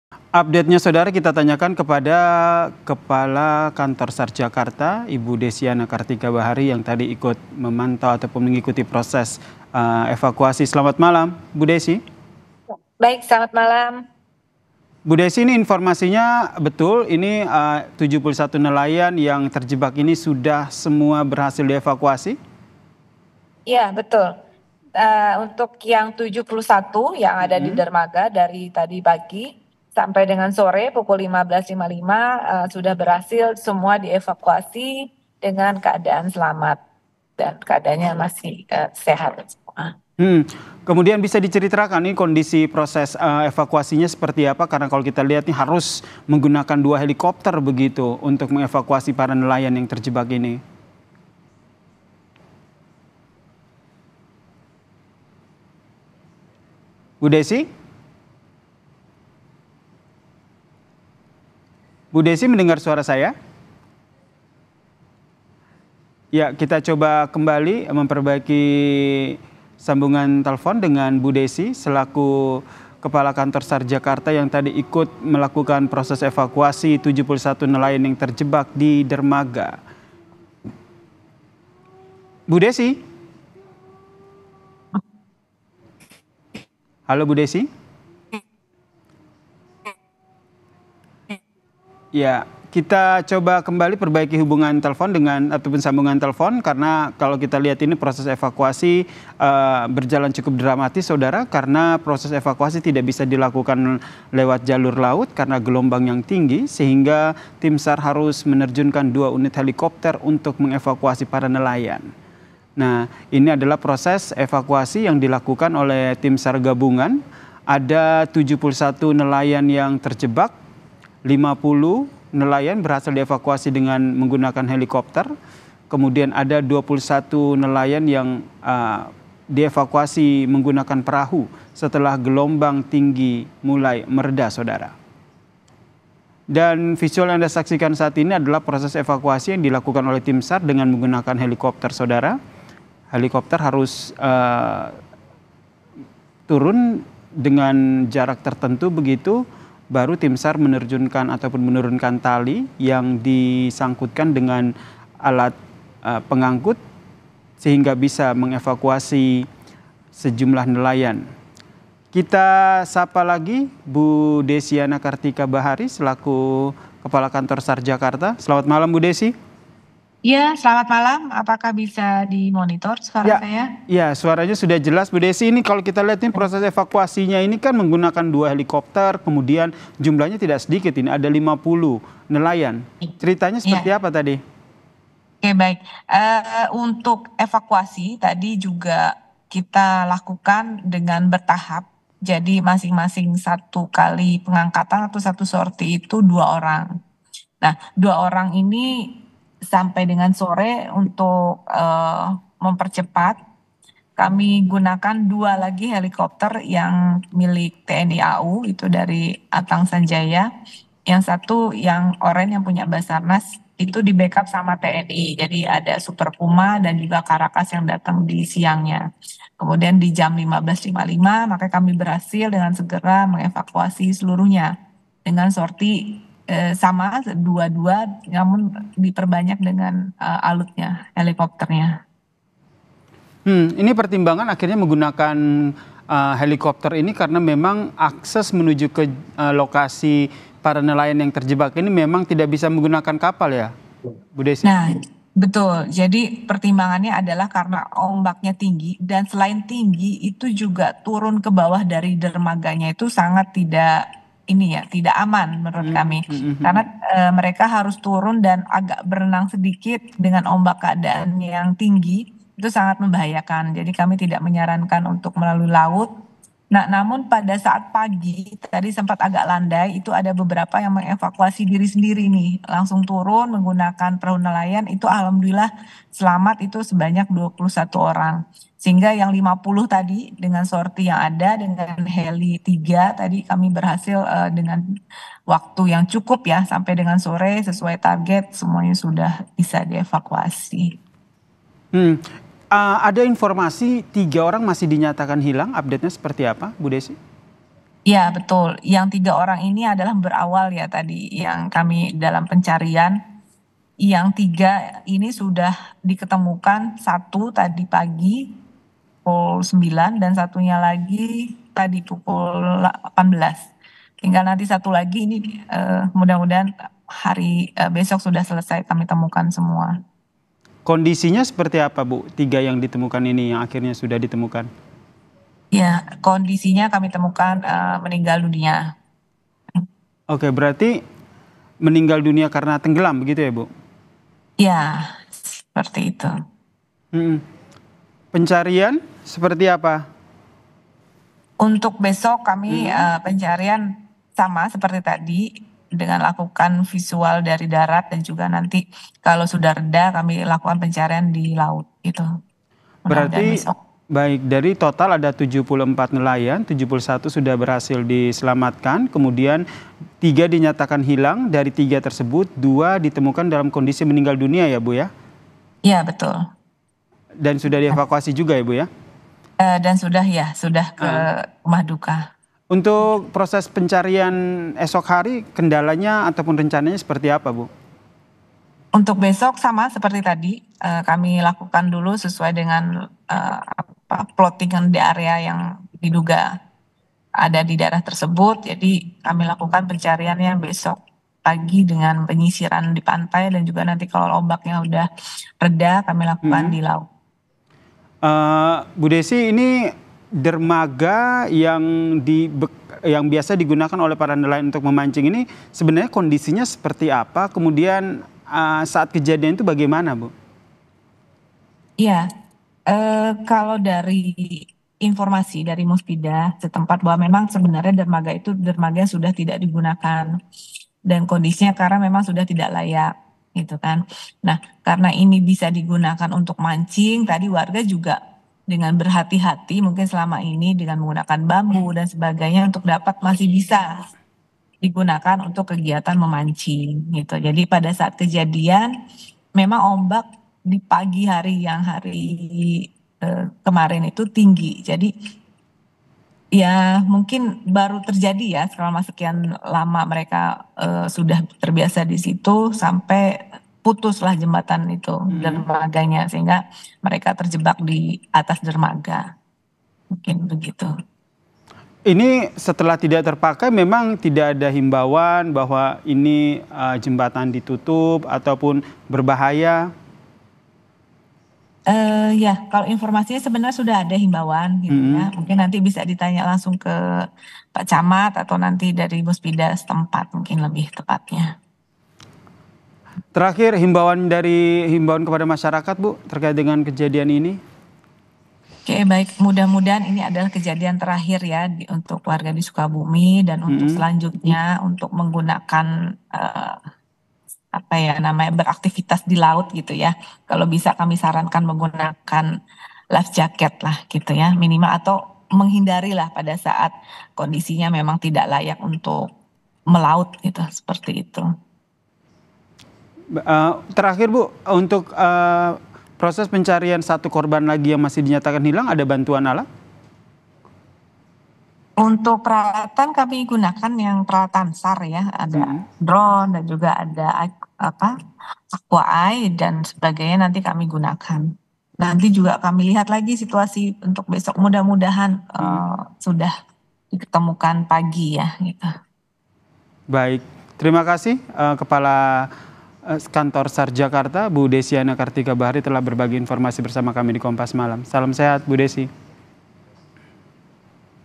Terungkap Ini Kronologi hingga Proses Evakuasi 71 Nelayan Terjebak di 'Bekas' Dermaga
JAWA BARAT, KOMPAS.TV - Gelombang tinggi yang menghantam dermaga di Pantai Tegal Buleud, Kabupaten Sukabumi, Jawa Barat, membuat para nelayan yang sedang memancing menjadi panik.

Gelombang besar tersebut menyebabkan akses jalan menuju dermaga terputus.

Akibatnya, 74 nelayan terjebak karena tidak dapat menyeberang kembali ke bibir pantai. Mereka hanya bisa pasrah menunggu bantuan.

Kejadian bermula saat 74 nelayan pergi memancing di bekas dermaga tambang pasir besi di wilayah Pantai Tegal Buleud, Kabupaten Sukabumi pada Selasa malam. Jembatan dermaga ini sudah tidak terpakai lagi dan merupakan bekas tambang pasir besi. Ketika para nelayan sedang memancing di ujung dermaga, tiba-tiba gelombang besar menerjang sehingga membuat jembatan roboh.

Akibatnya, akses menuju bibir pantai terputus, dan total 71 nelayan terjebak di dermaga, sementara 3 nelayan dinyatakan hilang.

Untuk pembaruan lebih lanjut terkait evakuasi para nelayan yang terjebak di dermaga Pantai Tegal Buleud, Kabupaten Sukabumi, simak penjelasan selengkapnya dari Kantor SAR Jakarta melalui Desiana Kartika Bahari.